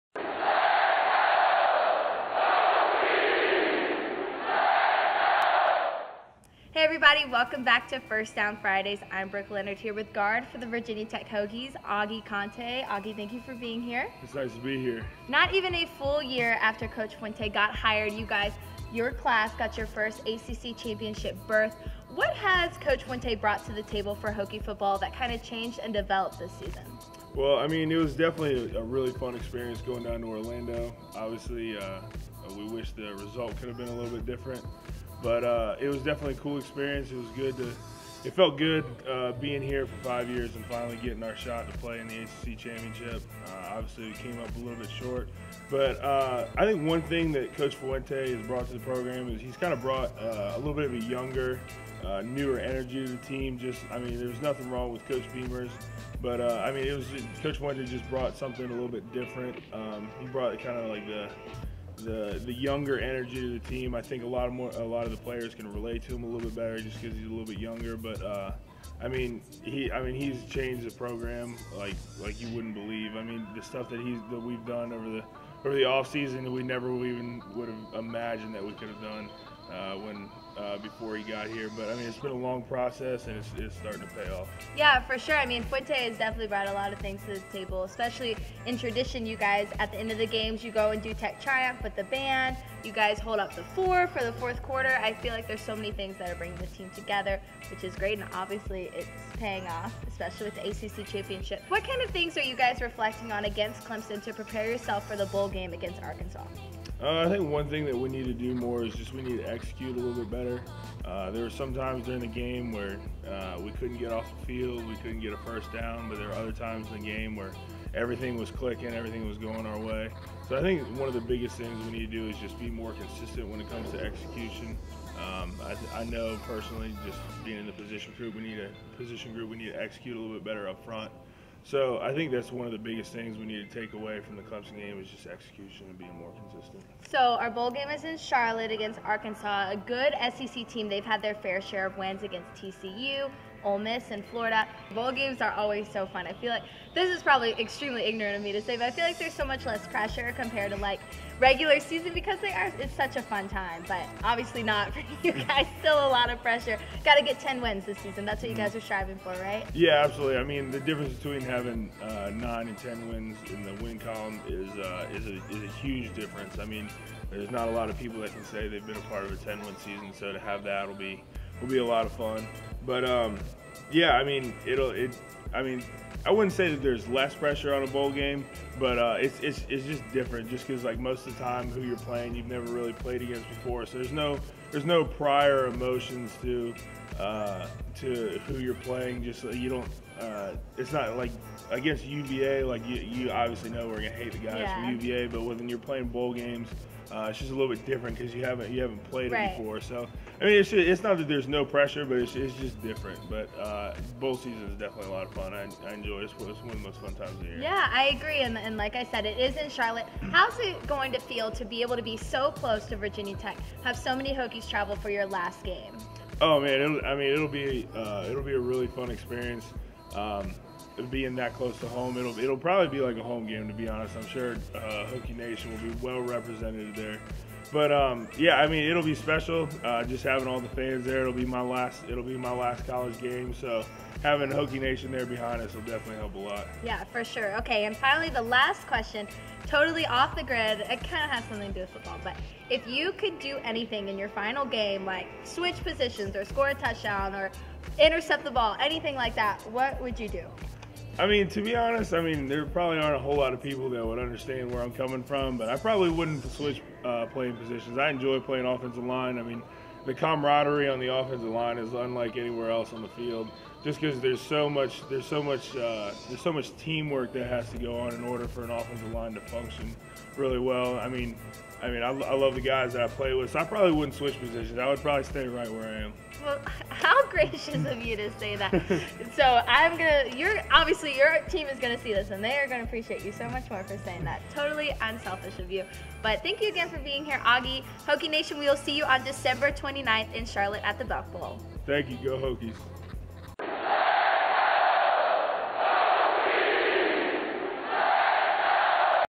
First Down Fridays: Augie Conte
I sit down with guard for the Virginia Tech Hokies, Augie Conte. We talk the ACC Championship, Justin Fuente, and the Belk Bowl.

Video: Stephen Dixon, HokieVision, Mark Umansky